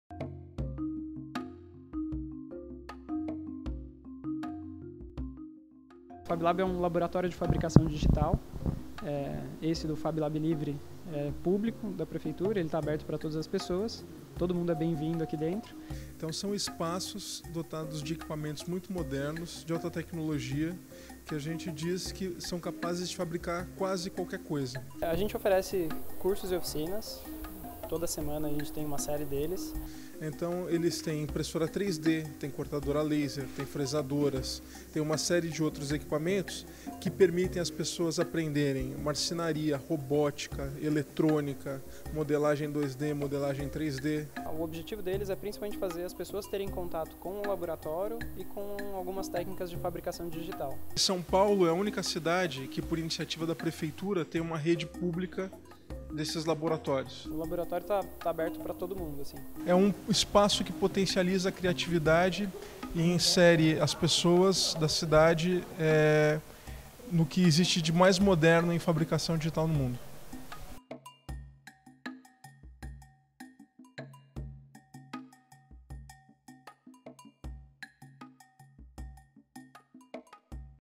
Conheça o Fablab SP da Prefeitura de São Paulo
Conheça o Fablab - Olido da Prefeitura de São Paulo, uma iniciativa da gestão Fernando Haddad. Trata-se de um laboratório de fabricação digital totalmente gratuito e aberto a população. Aperte o play e saiba mais.